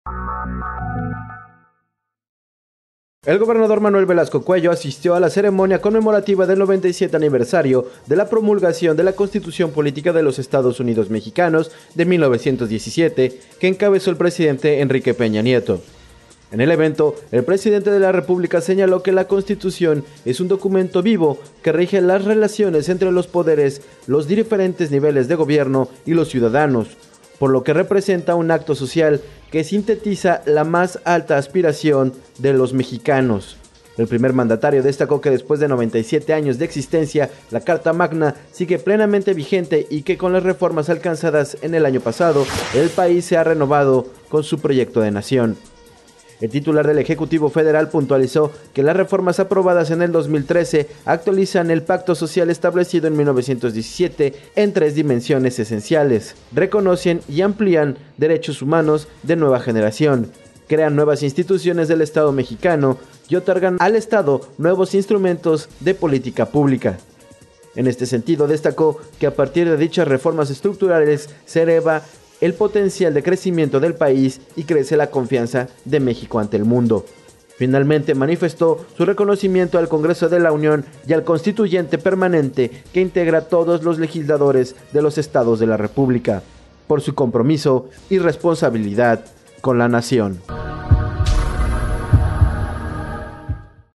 Asiste MVC a ceremonia conmemorativa de la promulgación de la Constitución
El gobernador Manuel Velasco Coello asistió a la ceremonia conmemorativa del 97 aniversario de la promulgación de la Constitución Política de los Estados Unidos Mexicanos de 1917, que encabezó el presidente Enrique Peña Nieto.

En el evento, el Presidente de la República señaló que la Constitución es un documento vivo, que rige las relaciones entre los Poderes, los diferentes niveles de gobierno y los ciudadanos, por lo que representa un pacto social que sintetiza las más altas aspiraciones de las y los mexicanos.